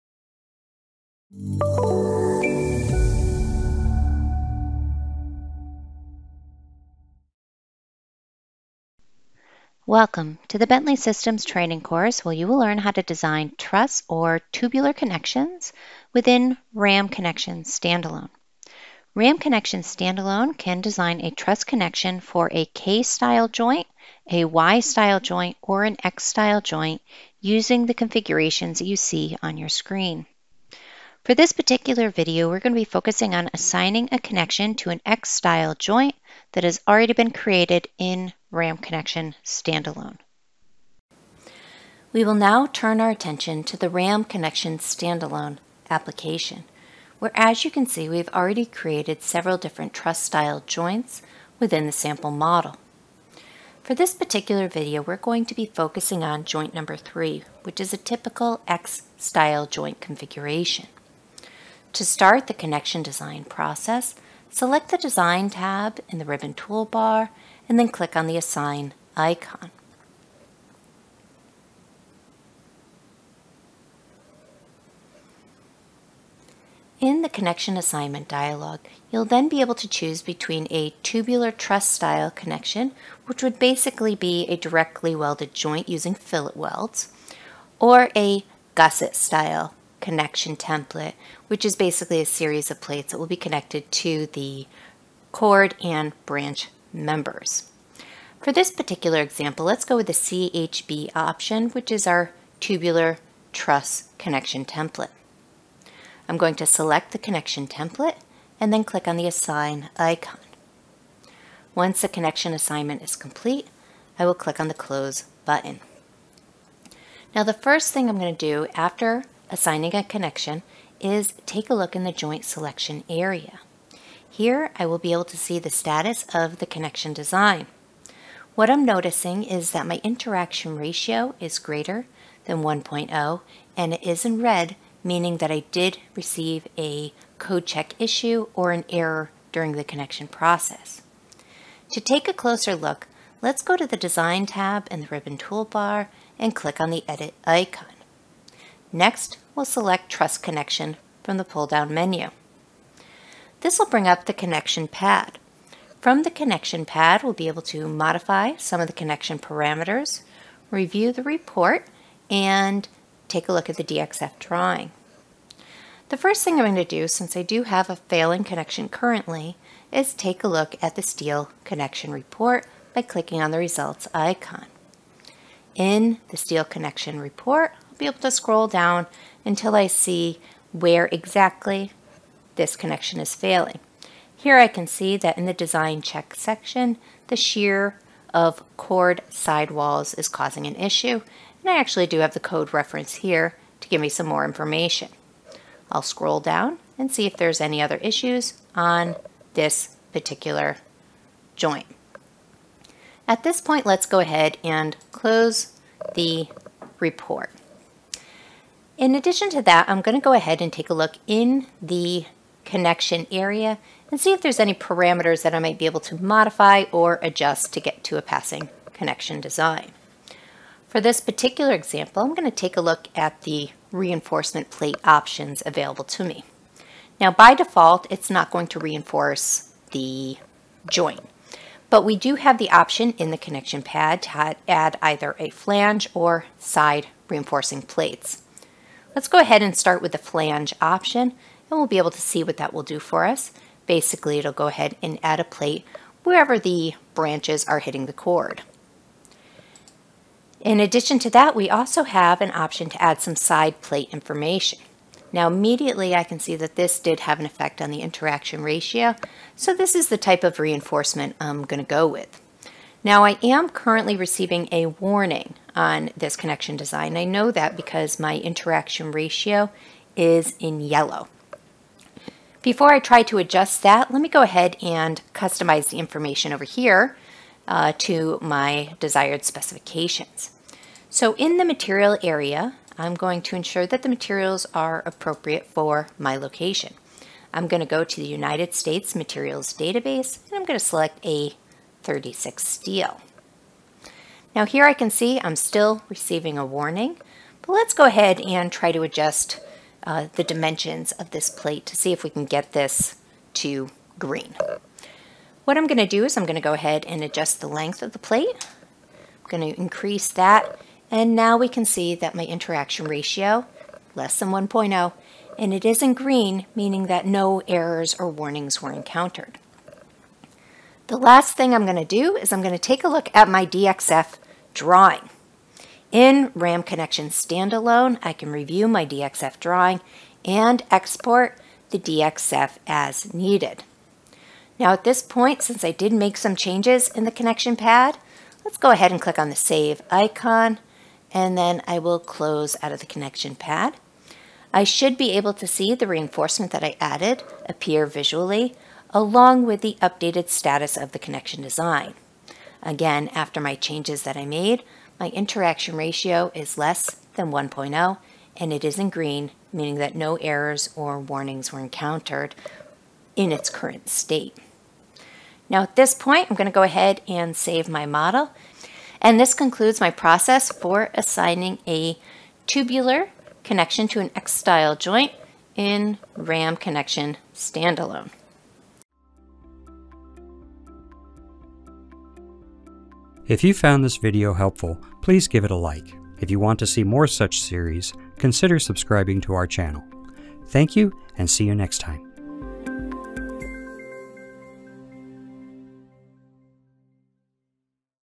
Designing Truss Connections for X-Joints in RAM Connection
In this video, you will learn how to design truss connections for X-Joints in RAM Connection.